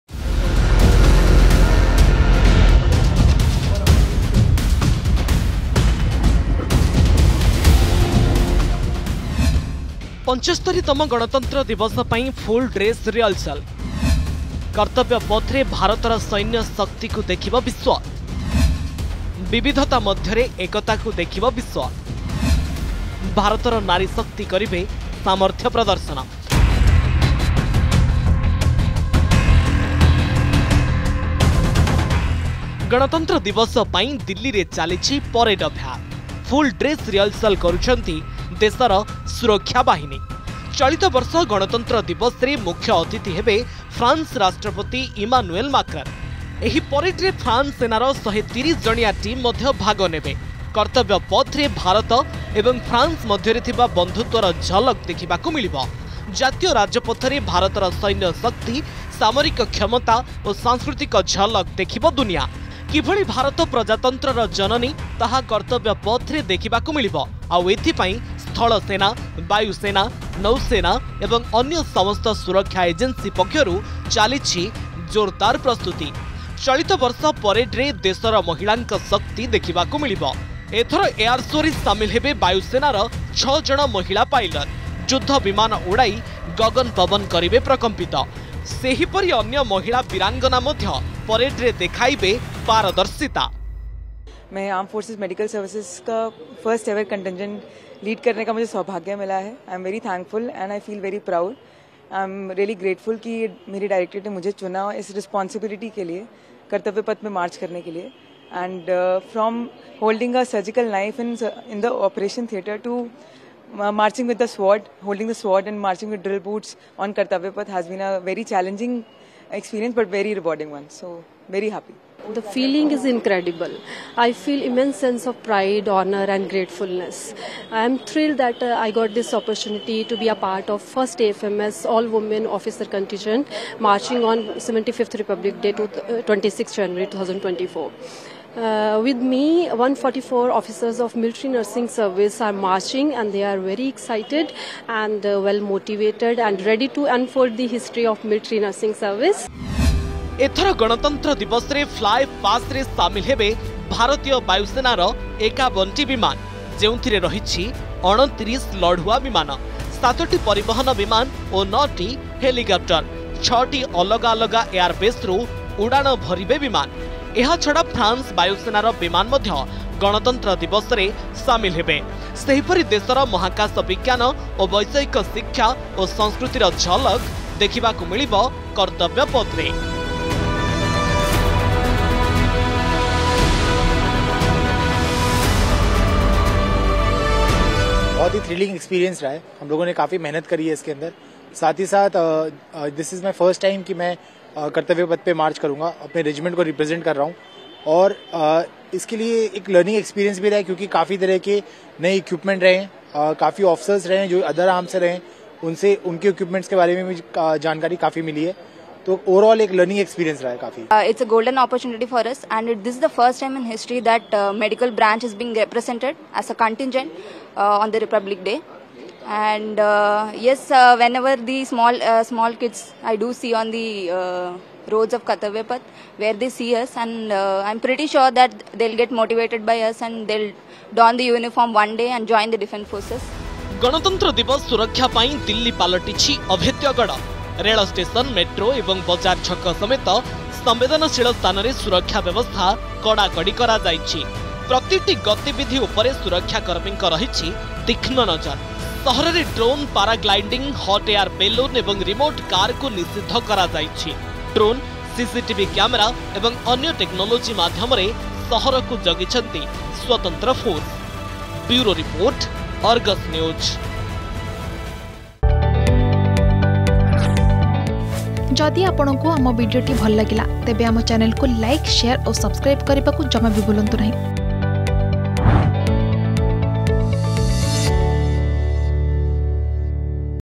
୭୫ତମ ଗଣତନ୍ତ୍ର ଦିବସ ପାଇଁ ଫୁଲ୍‌ ଡ୍ରେସ୍‌ ରିହର୍ସଲ୍‌ || 75th Republic Day || Full Dress Rehearsal
୭୫ତମ ଗଣତନ୍ତ୍ର ଦିବସ ପାଇଁ ଫୁଲ୍‌ ଡ୍ରେସ୍‌ ରିହର୍ସଲ୍‌ । କର୍ତ୍ତବ୍ୟପଥରେ ଭାରତର ସୈନ୍ୟ ଶକ୍ତିକୁ ଦେଖିବ ବିଶ୍ବ । ବିବିଧତା ମଧ୍ୟରେ ଏକତାକୁ ଦେଖିବ ବିଶ୍ବ । ଭାରତର ନାରୀ ଶକ୍ତି କରିବେ ସାମର୍ଥ୍ୟ ପ୍ରଦର୍ଶନ ।

୭୫ତମ ଗଣତନ୍ତ୍ର ଦିବସ ପାଇଁ ଫୁଲ୍‌ ଡ୍ରେସ୍‌ ରିହର୍ସଲ୍‌ || 75th Republic Day || Full Dress Rehearsal